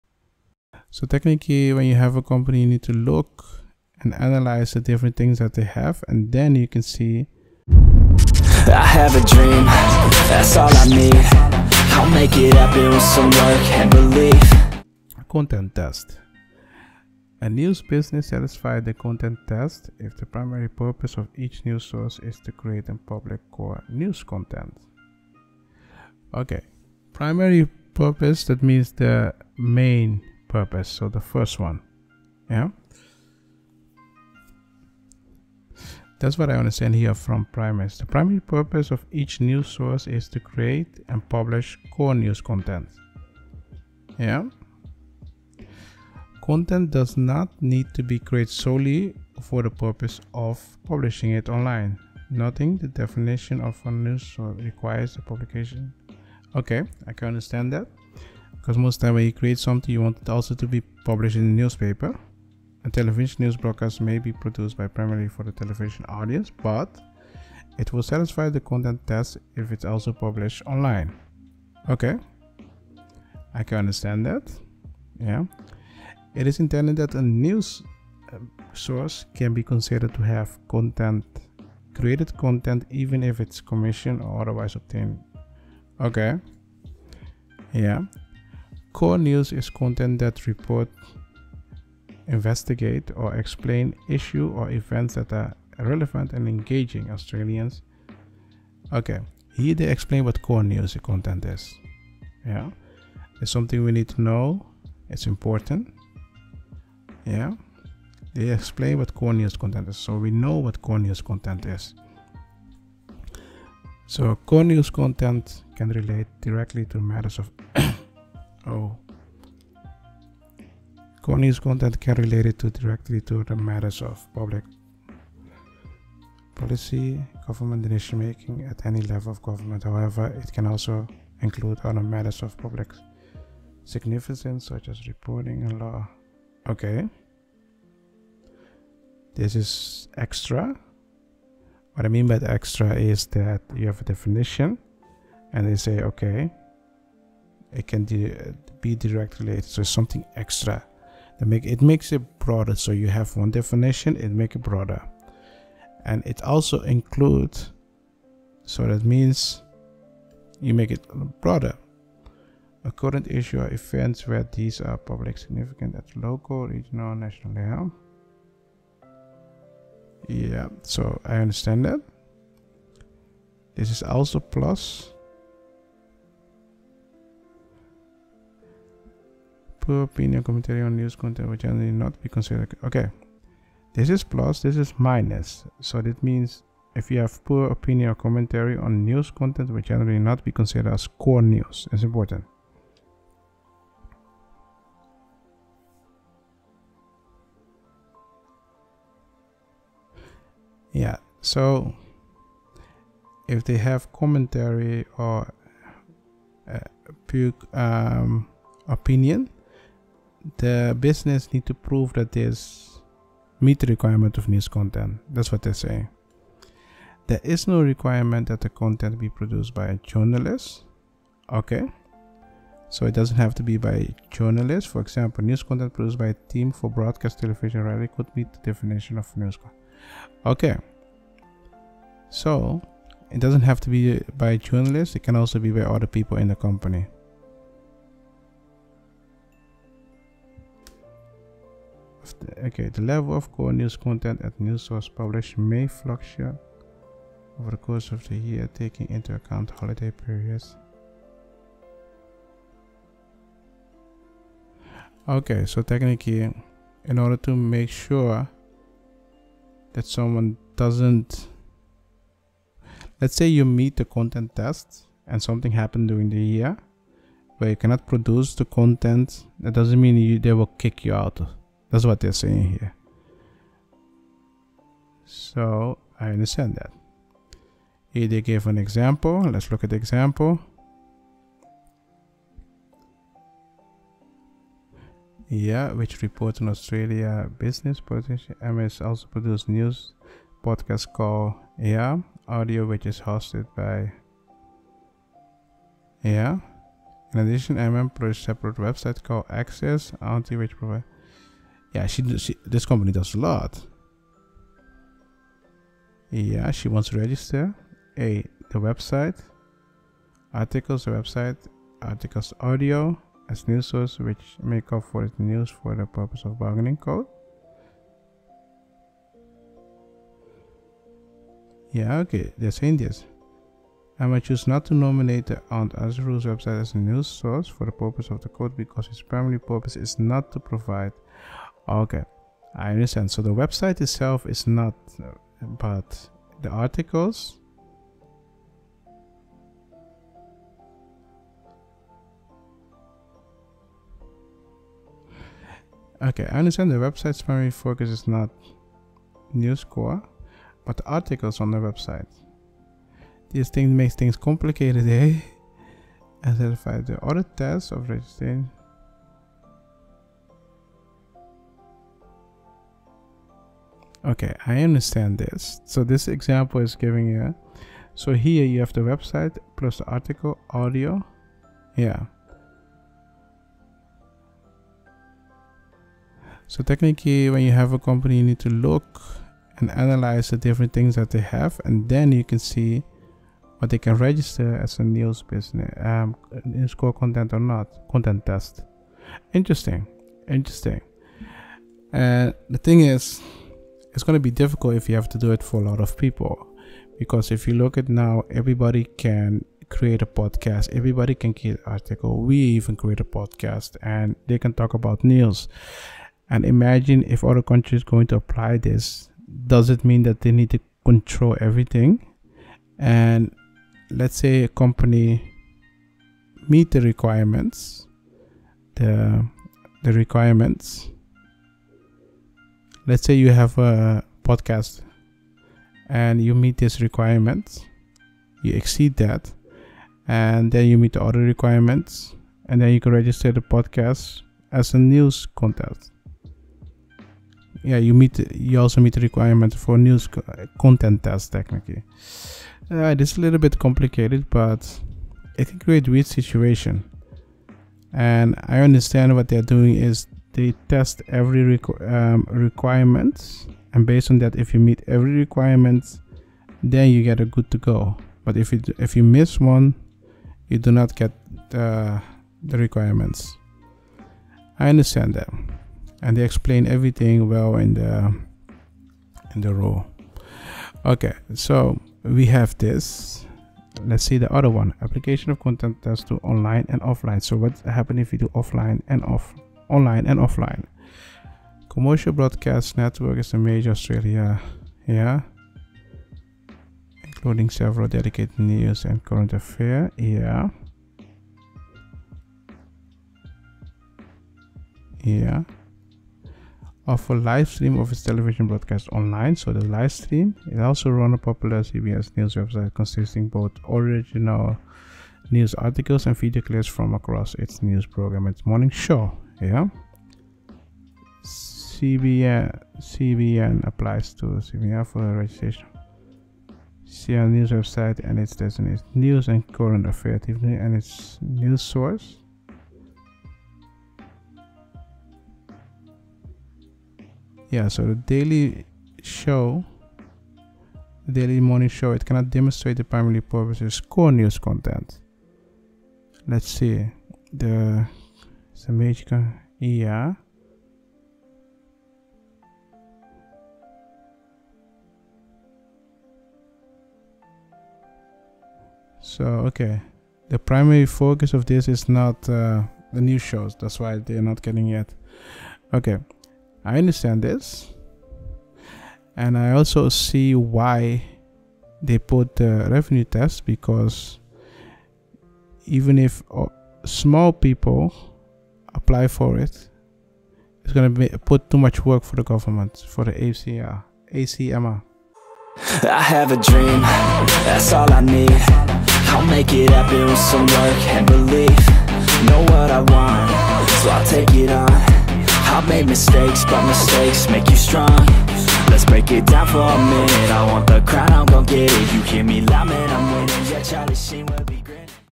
The Content Test? | Martisz [ Australia Media law ]
Australia has passed a new law that will require digital platforms like Facebook and Google to pay local media outlets and publishers to link their content in news feeds or search results.